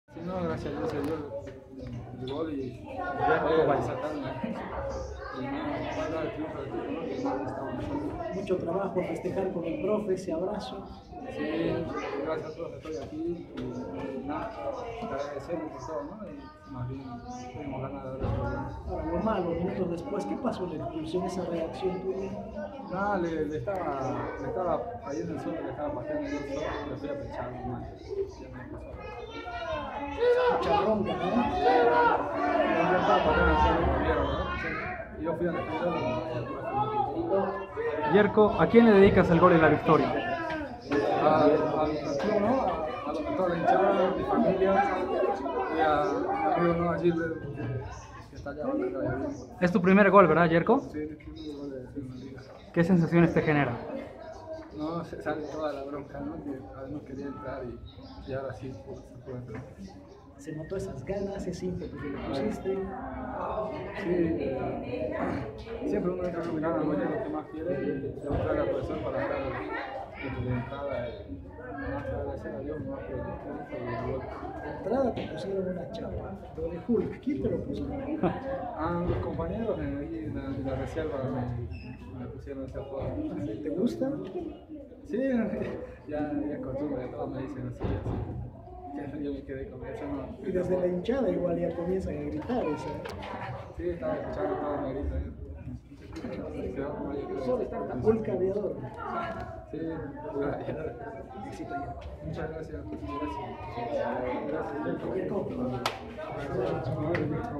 Yerco Vallejos 25 de Agosto del 2018
Declaraciones del delantero Yerco "Hulk" Vallejos sobre los siguientes temas:
Victoria frente a Royal Pari.